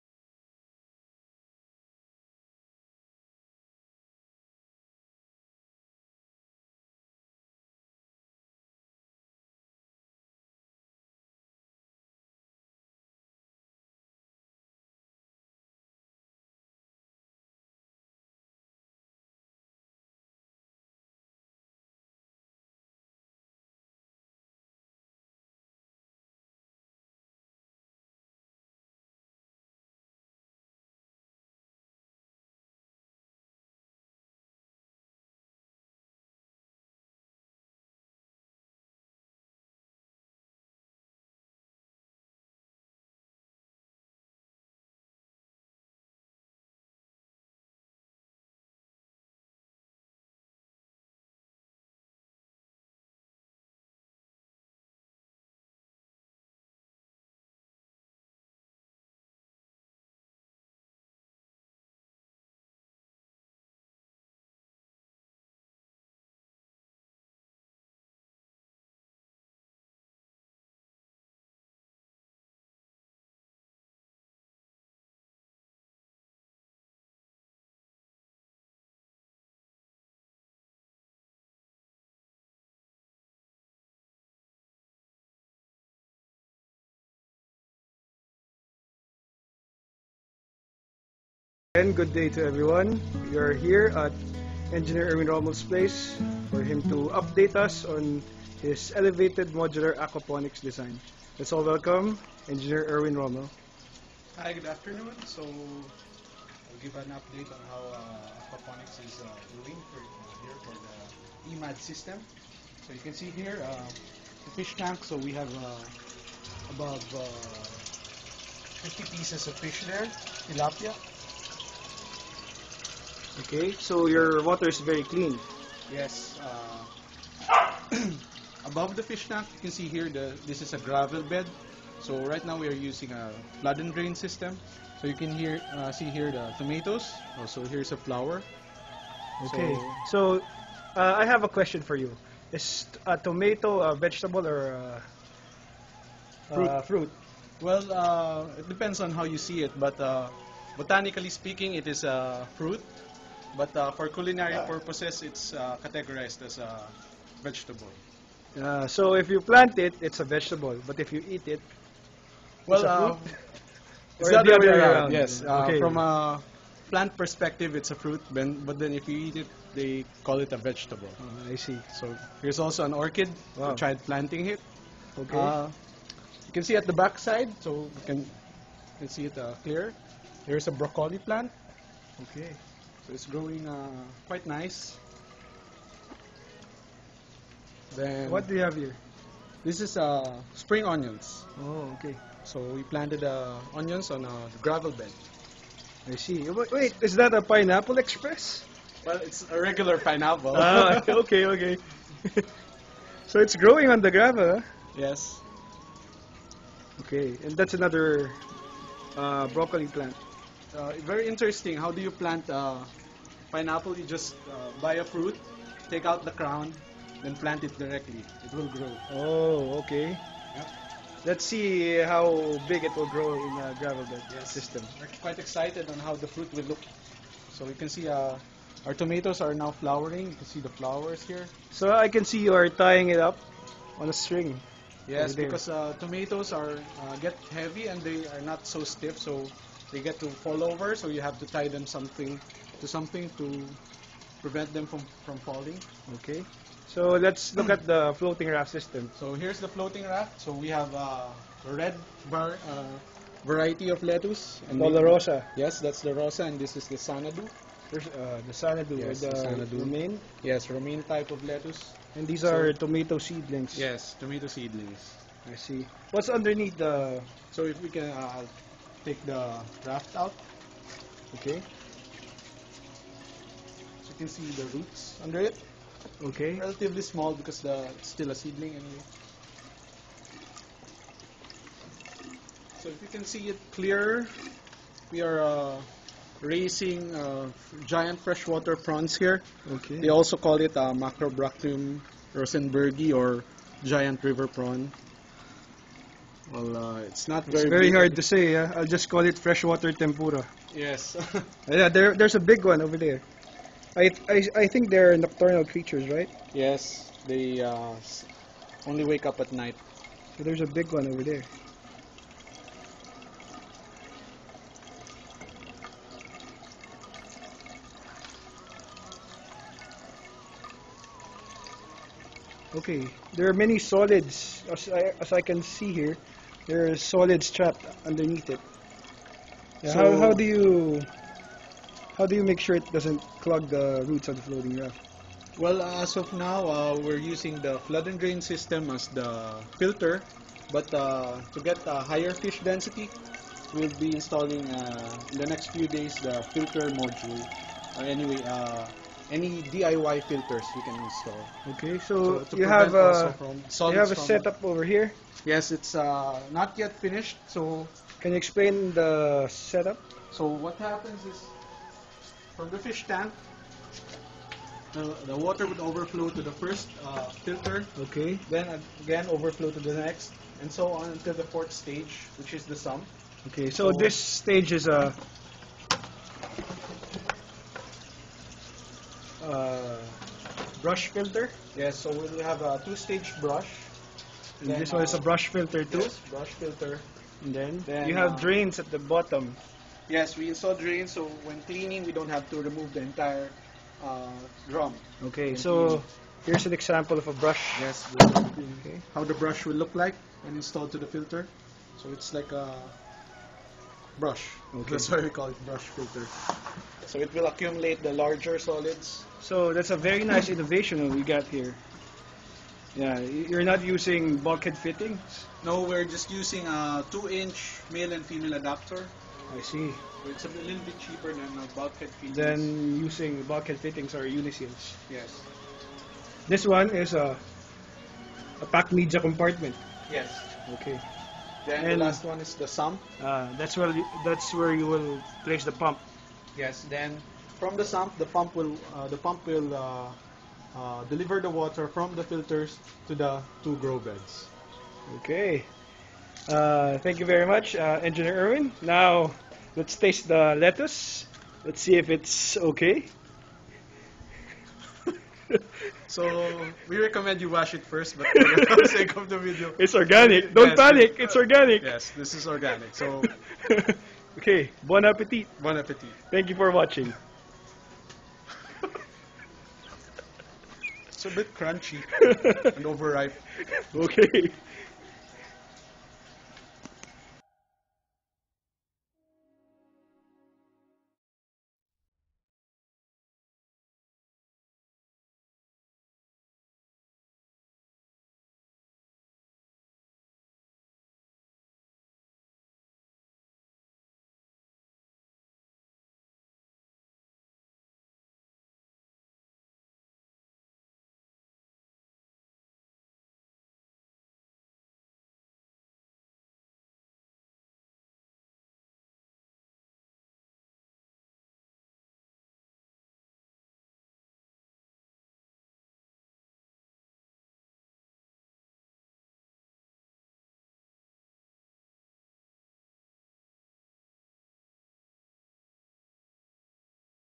How the Elevated Growbed EMAD Aquaponics works, MADE in the Philippines
Modular Aquaponics Design and Equipment

Elevated Modular Aquaponic Design (EMAD)

Visiting the system of Engr. Erwin Rommel.

EMAD Update